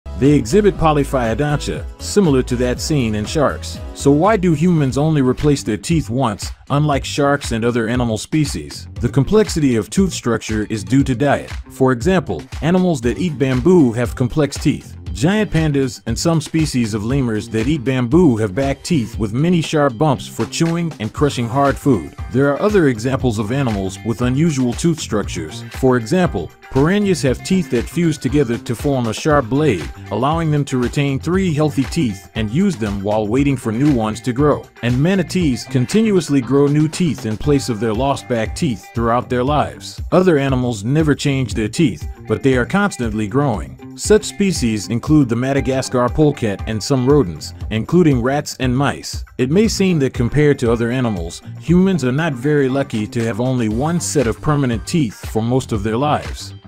Why animals Have Regenerating Teeth and Humans don't Drugs for Growing New Teeth
The Future of Humanity: japan has almost created drugs that can regrow missing teeth! A Japanese research team is currently working on a groundbreaking drug that promises to grow new teeth for people. Clinical trials for this tooth re-growth drug are due to begin in July 2024, with the aim of making it available for general use by 2030.

For further information or inquiries about Takahashi's research, please (in Japanese)